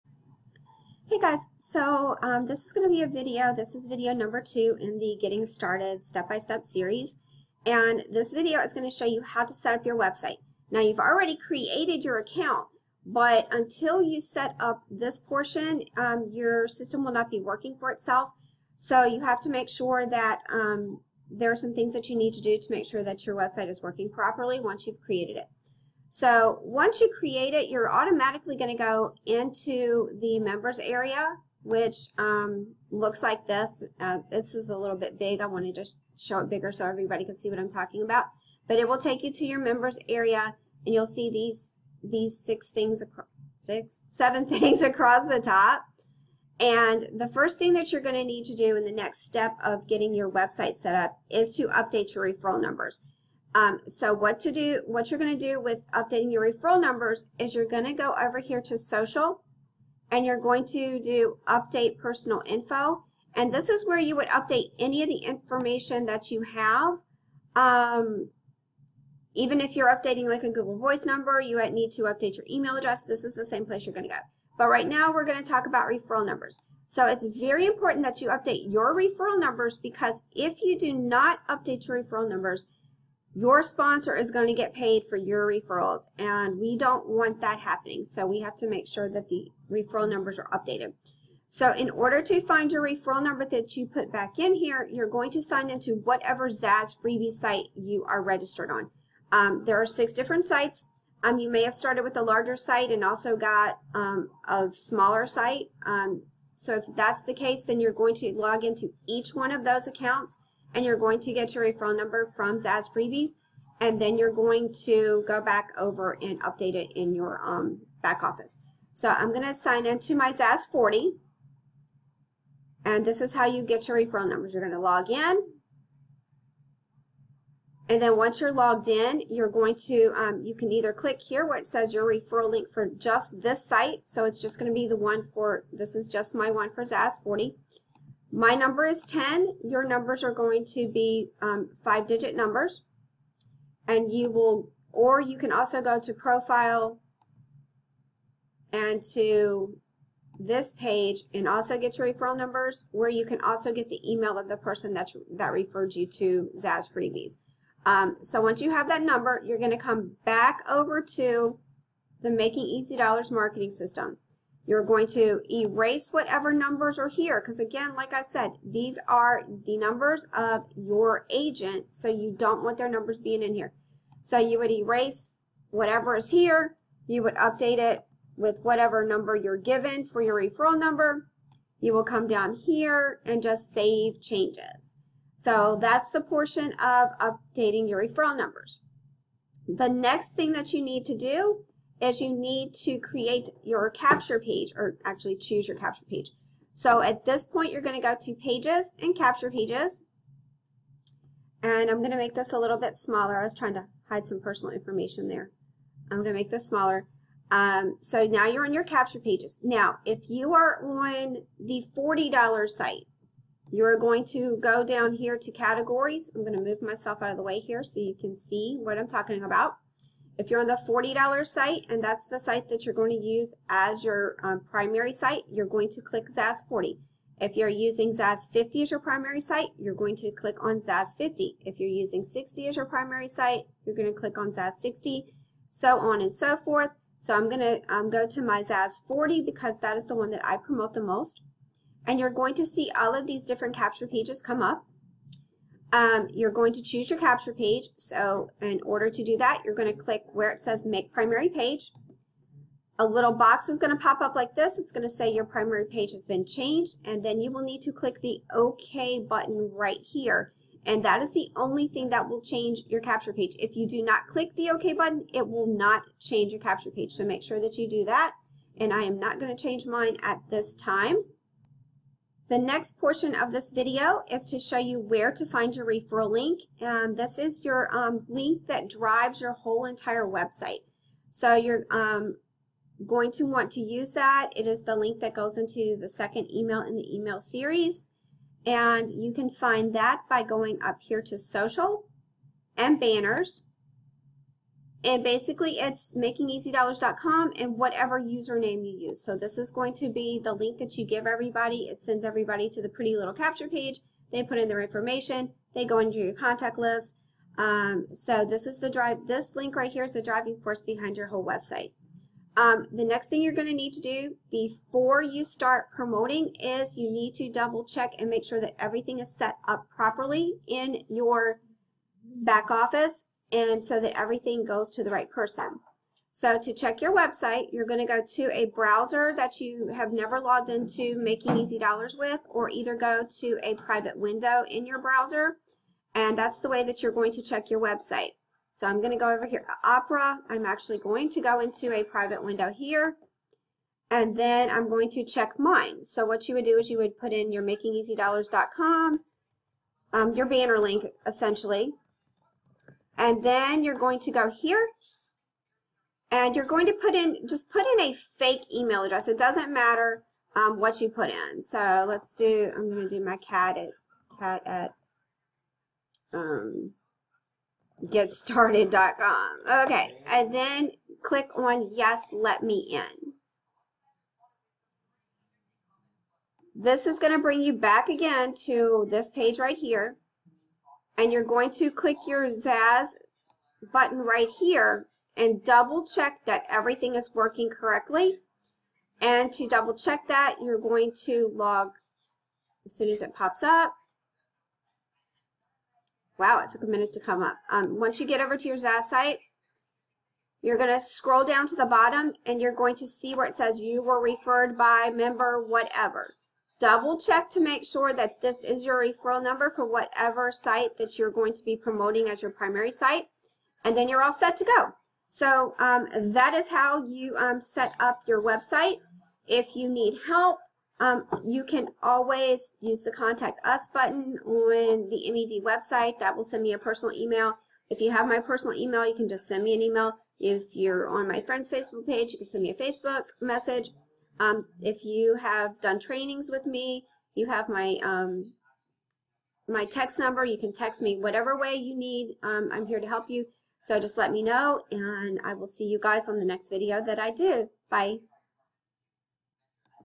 Making Easy Dollars - Setting Up Your Website (Updated 9.2.19)
This video goes through the process of setting up your website on Making Easy Dollars.